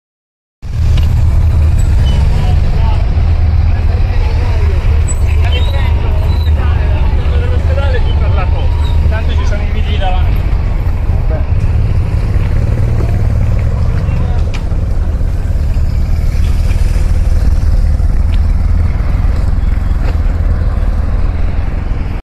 Fiaccola della Pace, comincia il viaggio verso Cascia
La partenza per la prima tappa, Arquata del Tronto. Domani l'arrivo in Valnerina